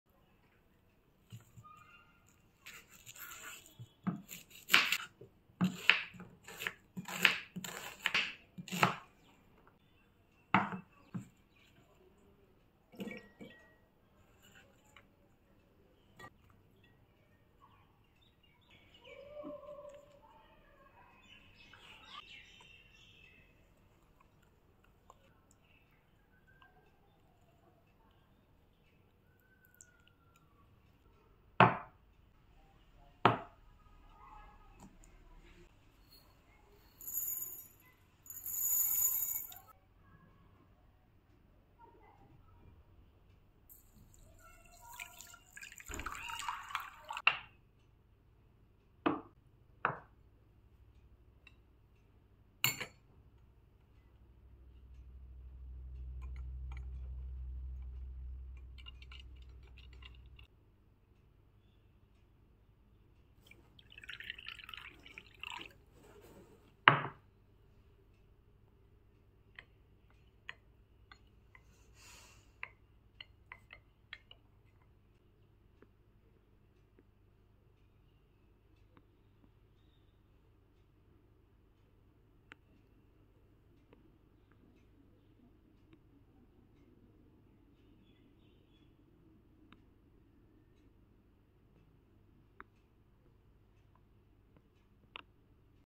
Regrow Hairs Naturally Using Onion Juice
There are many home remedies of onion juice to regrow hairs but this one will also add a boost of shine and improve the texture of hairs too.